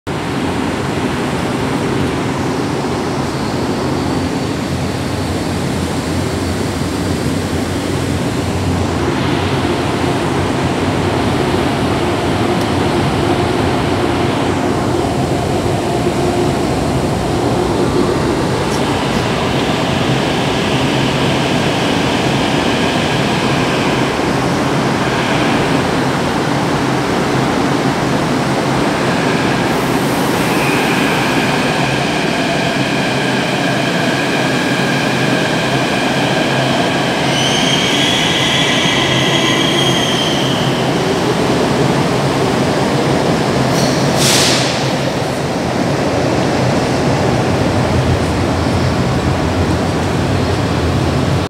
Série 500 (532 e 511) na estação Santa Cruz
Frota P/Série 500 (P32/532 e P11/511) (CAF)

532 partindo sentido Capão Redondo
511 chegando sentido Chácara Klabin